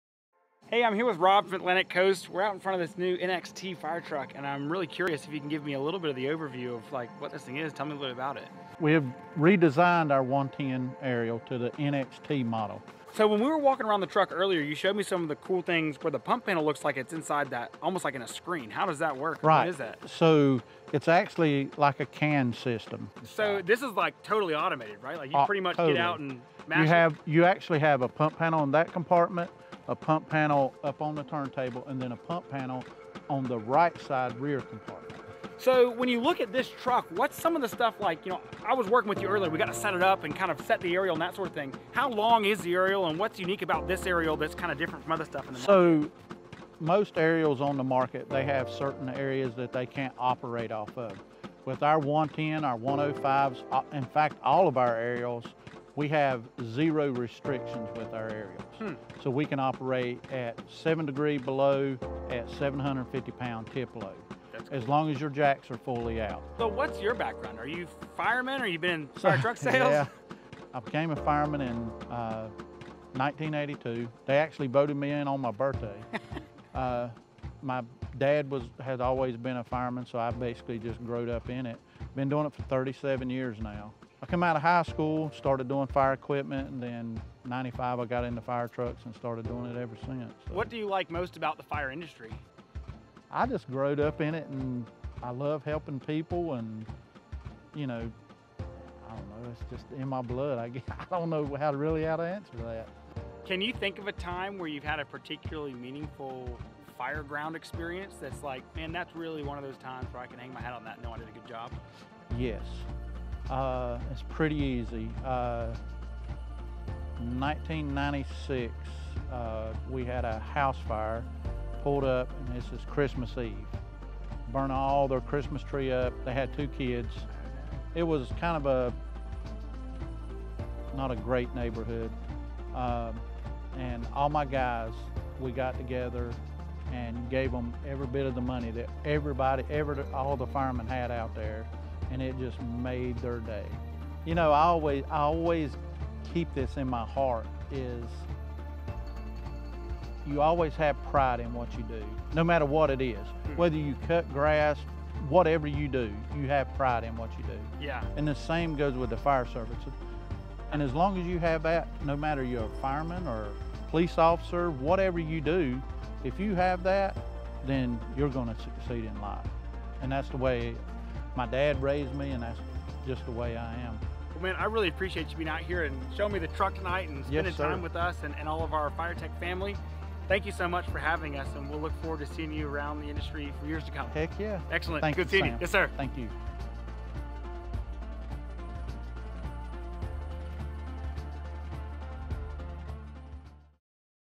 Spartan NXT110 with FireTech BG2 Lighting System
The Spartan Emergency Response NXT110 has some unique features and we're proud to show off a full BG2 Lighting System on this truck!

Rob Smith of Atlantic Coast Fire Trucks walks us through some of those features, and also talks a little bit about his career and what it's like to be a Firefighter.